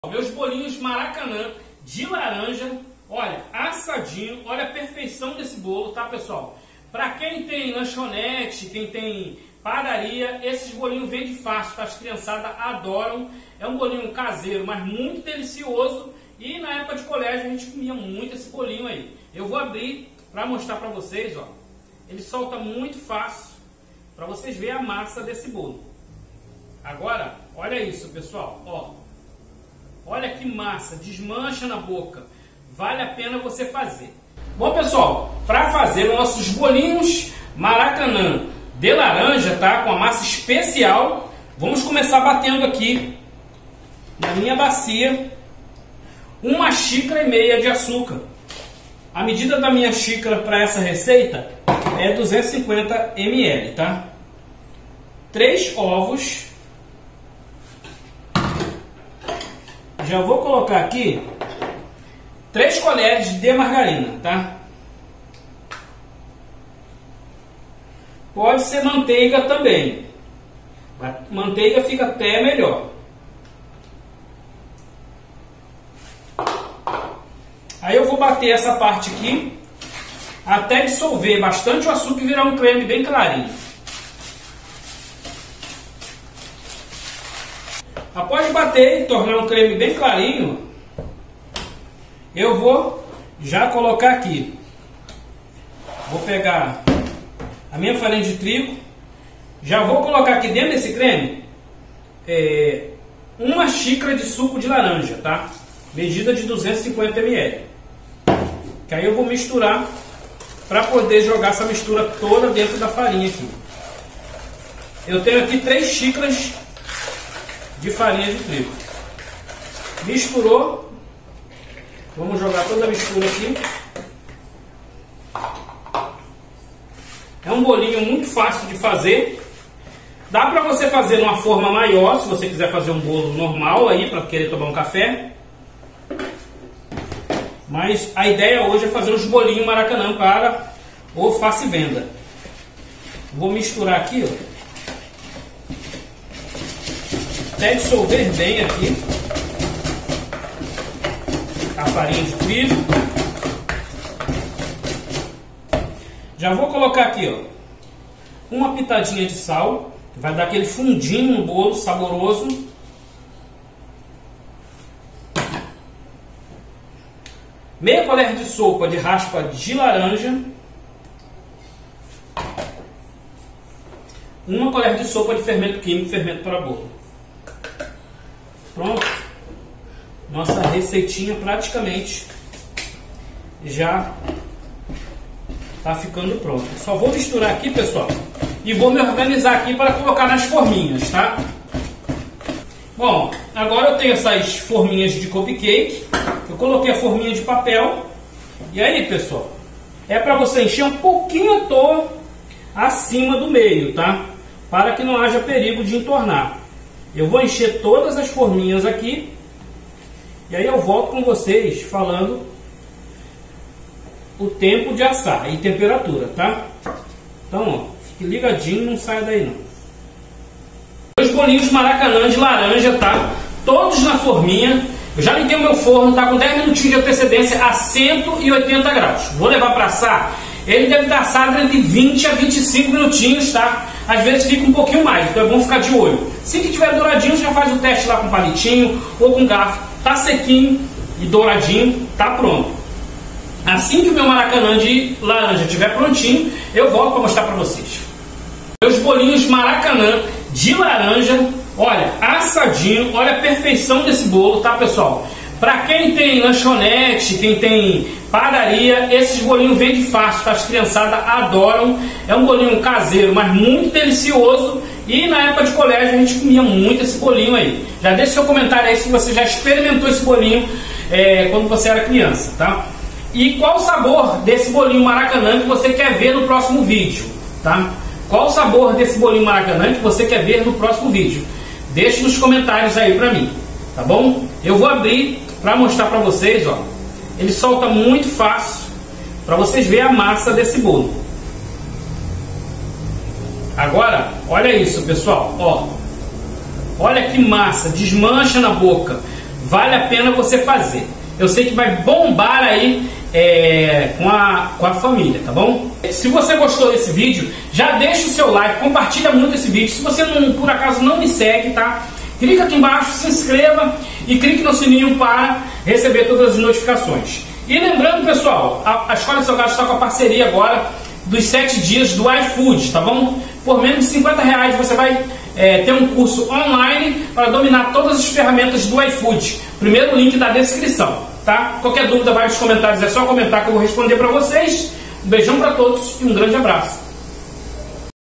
Bolinhos de saia sabor laranja
Impulsione seu negócio de culinária com nossas deliciosas receitas! Torne-se um especialista em fazer Bolinhos  sabor laranja com nosso tutorial passo a passo. 👇👇

Hoje, vamos aprender a fazer Bolinhos de  laranja. Uma receita simples e deliciosa que vai encantar a todos.

ingredientes

1 xicara e 1\2 de açúcar
3 ovos
3 colher de margarina
3 xicaras de farinha trigo
1 xicara de suco de laranja
1 pitada de sal
1\2 colher de raspas de laranja
1 colher de fermento para bolo